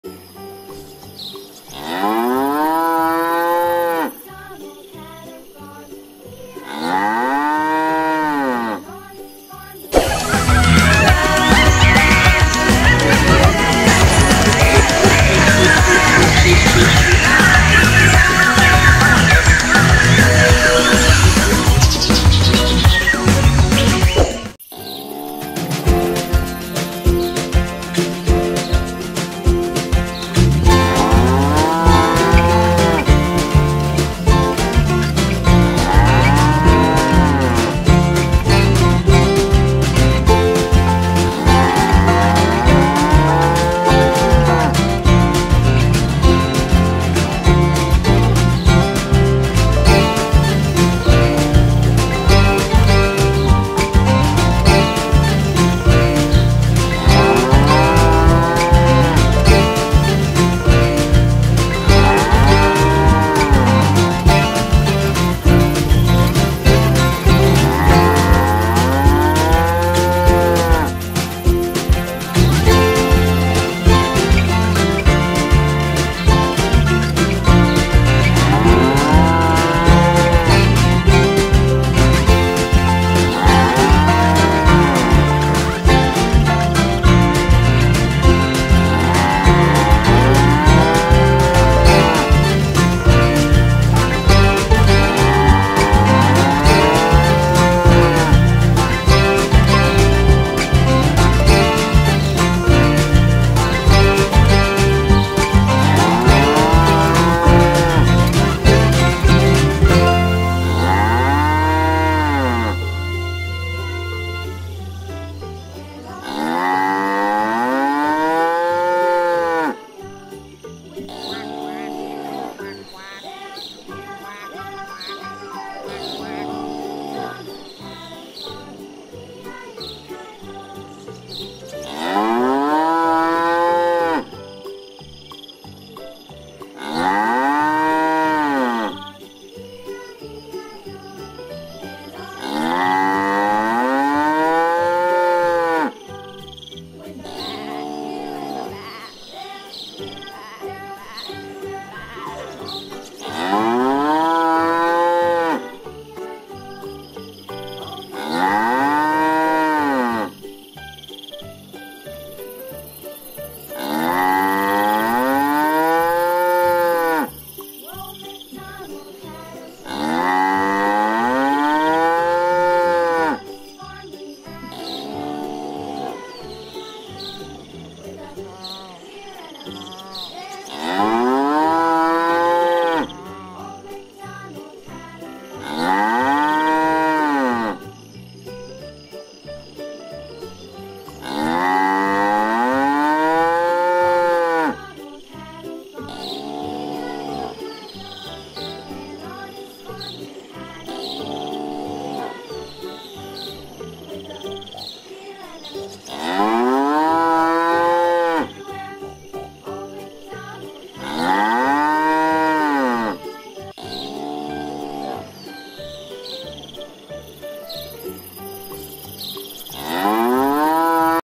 ANAK SAPI LUCU BERMAIN BERSAMA IBU NYA, SUARA SAPI, COW VIDEOS INDONESIA
Hewan ternak anak sapi lucu bersama ibunya bermain di sawah pedesaan jawa barat. Mooo suara sapi lucu ( cow videos Idonesia )